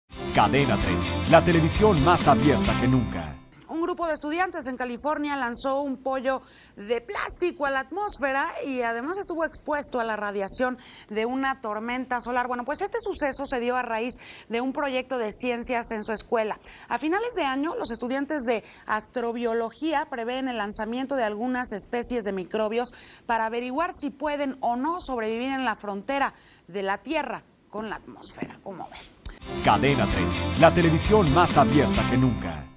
Camilla, el pollo de goma que viajó al espacio
Un grupo de estudiantes en California lanzó un pollo de plástico a la atmósfera, el ave de goma estuvo expuesto a la radiación